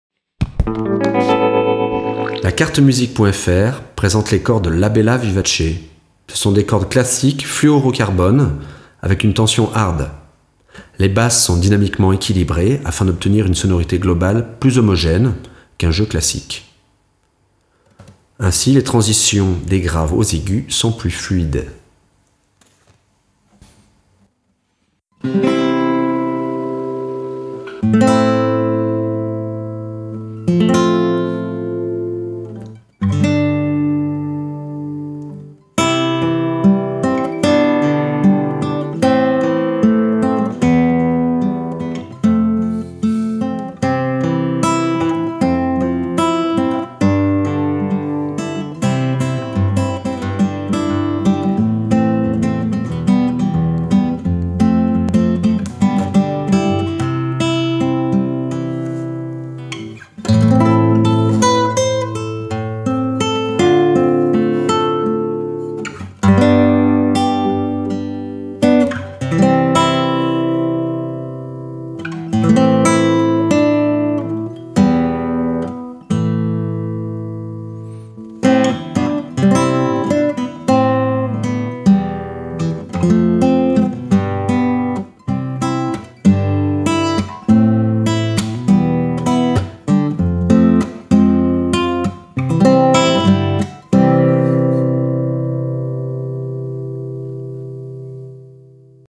Cordes de guitare Classique La Bella Vivace
Cordes de guitare classique fluorocarbone La Bella Vivace en tension forte.
Basses dynamiquement équilibrées, ce qui donne un ensemble homogène et fluide. La transition entre la corde de ré et de sol est d'une fluidité remarquable.